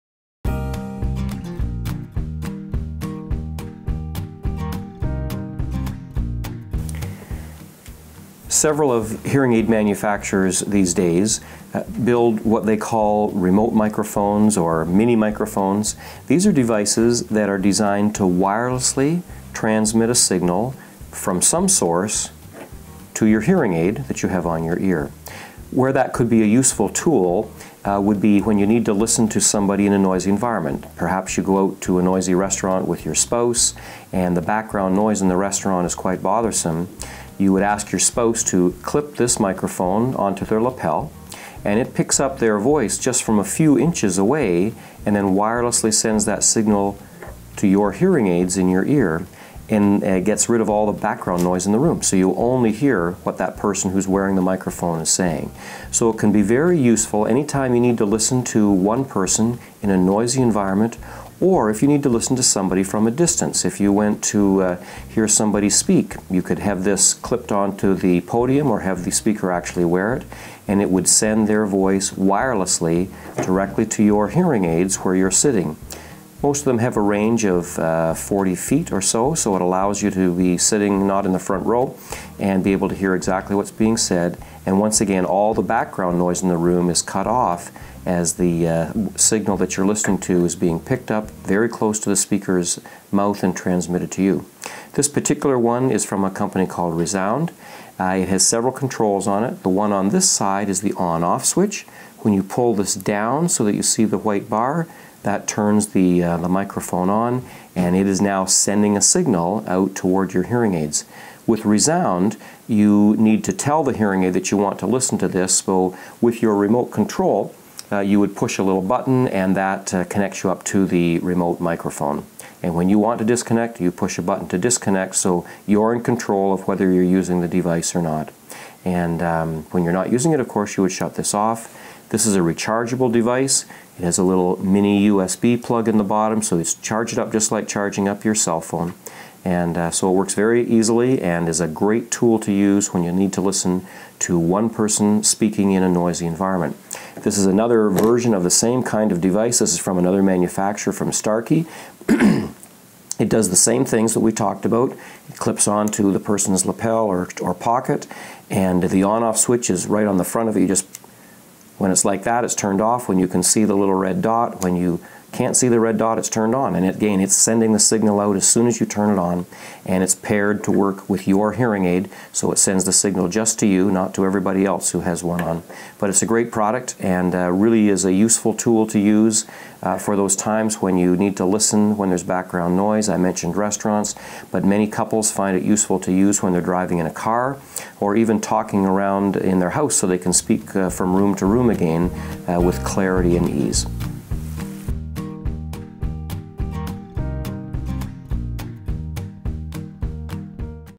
Remote Microphones for Hearing Aids
Brian Field, Doctor of Audiology describes what remote microphones for hearing aids are and what situations they are useful for. He also describes how remote microphones work, and demonstrates the controls on the microphones. This video features remote microphones from Resound and Starkey.